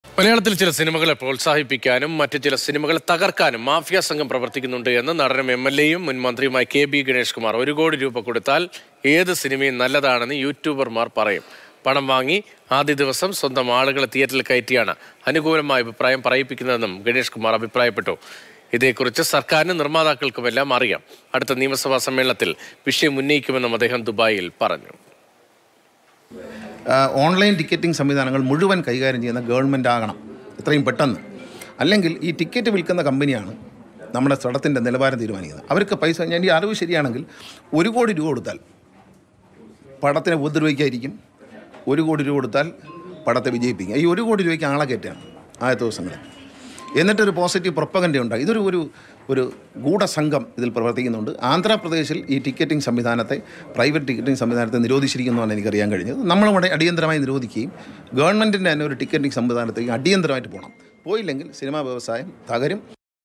മലയാളസിനിമകളെ തകർക്കാൻ ഗൂഢസംഘങ്ങൾ - കെ.ബി.ഗണേഷ് കുമാർ | K B Ganesh Kumar | Cinema
മലയാളത്തിൽ ചില സിനിമകളെ പ്രോത്സാഹിപ്പിക്കാനും മറ്റു ചില സിനിമകളെ തകർക്കാനും ഗൂഢസംഘങ്ങൾ പ്രവർത്തിക്കുന്നുണ്ടെന്ന് നടനും എംഎൽഎയുമായ കെ.ബി.ഗണേഷ് കുമാർ

Happy viewing!